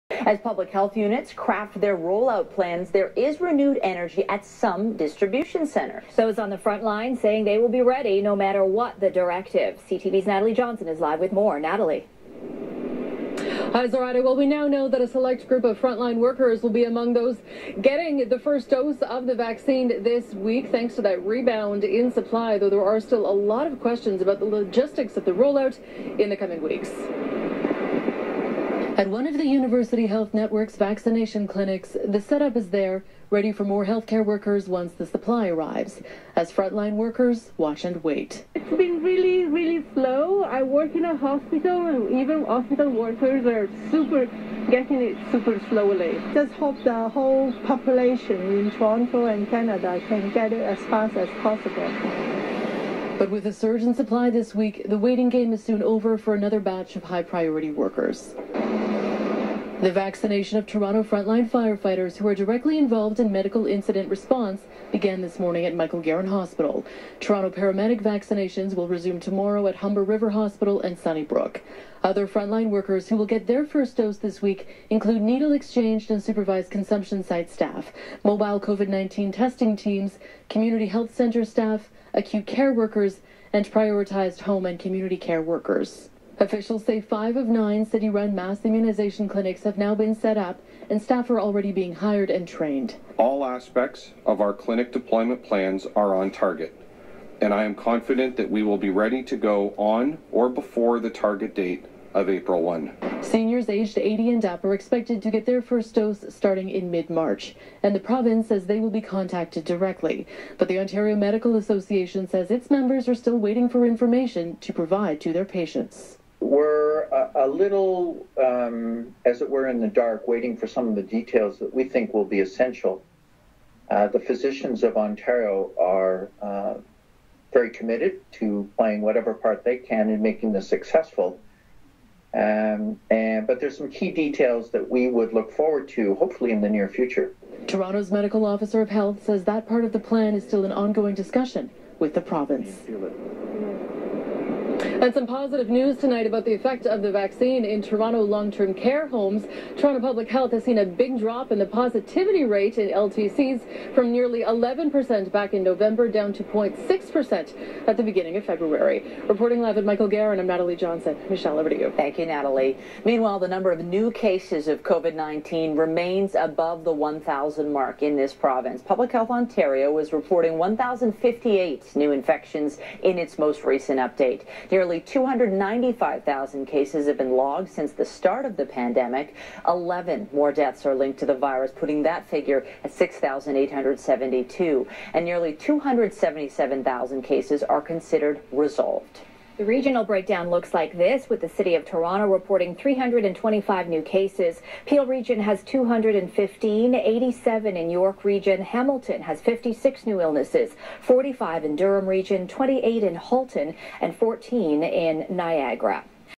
Some Front Line Workers are Ready for the Vaccination Rollout
The logistics of the rollout plan is not yet ready but the front line workers are.